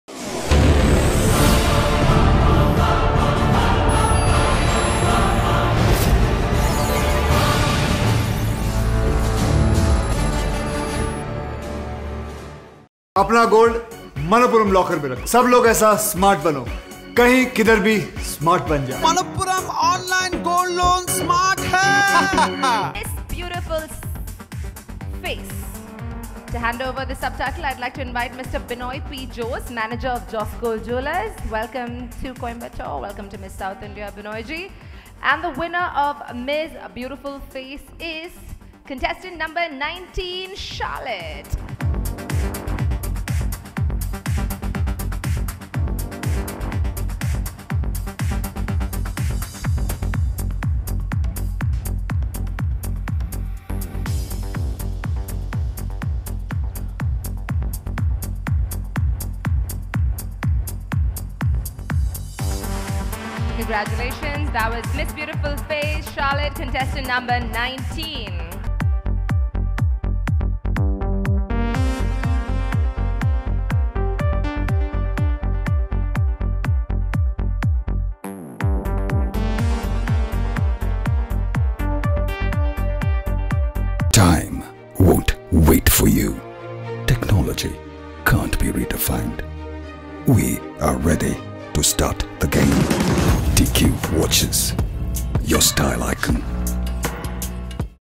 Miss South India 2019 - Miss Beautiful Face
Manappuram Miss South India 2019, held at Le Meradian Hotel , Coimbatore.  An event by Pegasus and conceived by Dr. Ajit Ravi Pegasus. Miss Beautiful Face was awarded to Sharlett Riya (Tamil Nadu) .The  Prize was handed over by Mr. Binoy P Jose sales Manager Josco Jewellers